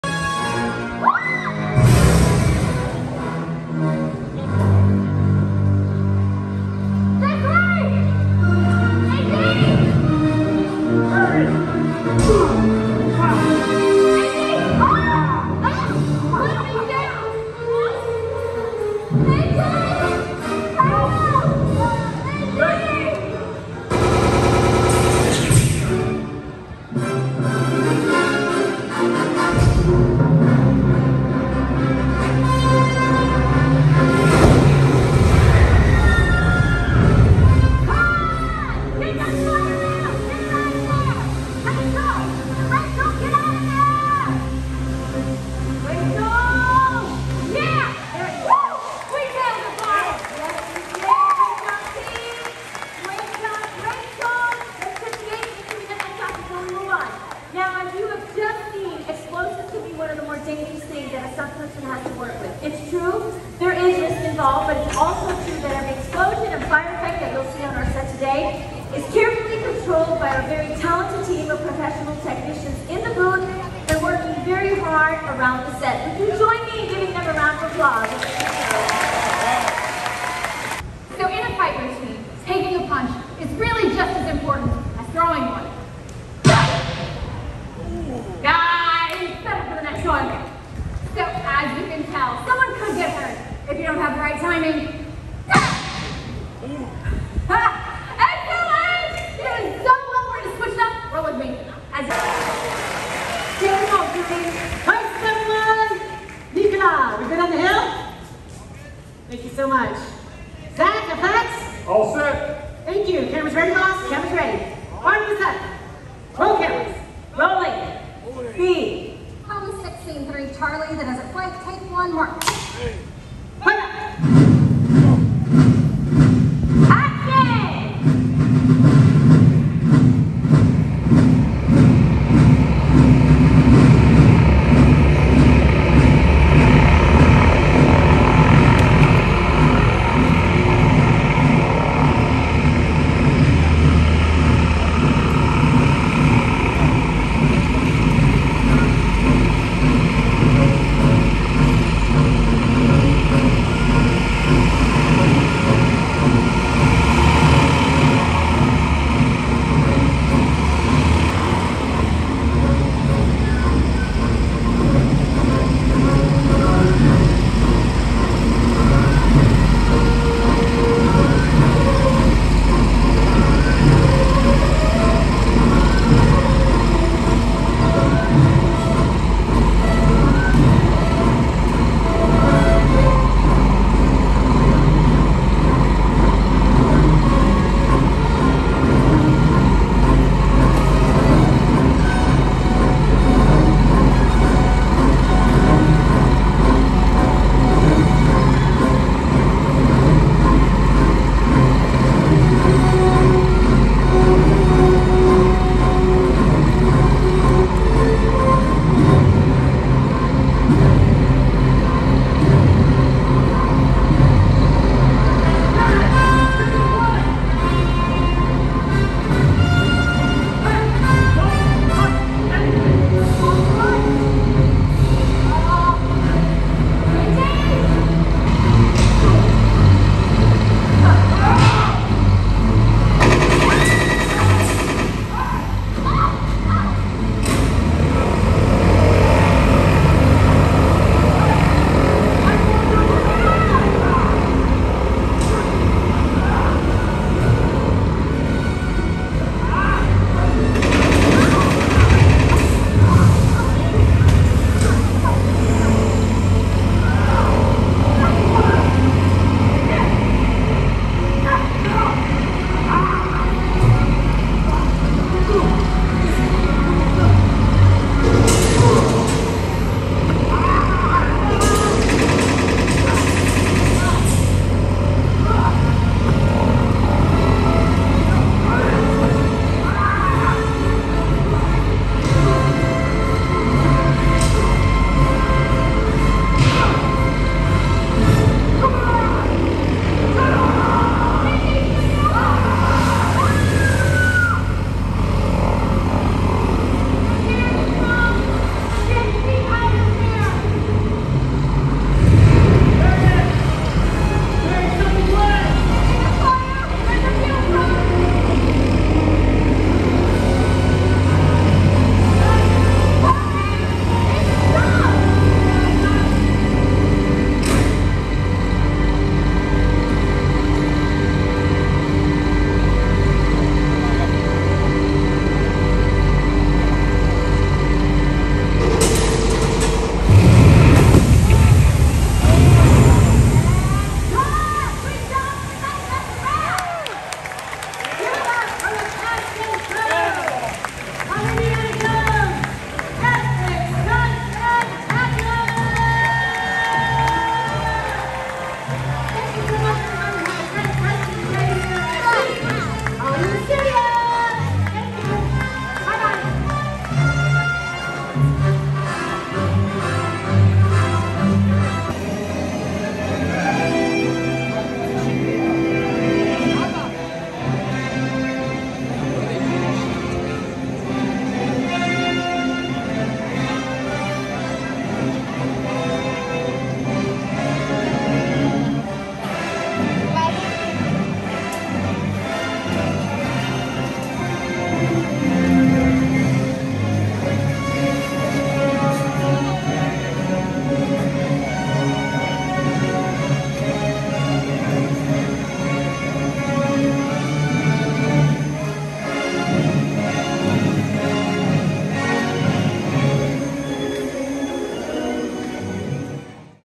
Indiana Jones Epic Stunt Spectacular Is Back at Disney’s Hollywood Studios
Wander With Us on to The Indiana Jones Epic Stunt Spectacular and check out a small preview of the Classic Attraction after a long closure due to the Pandemic!

It lets us know you are watching and enjoying our videos.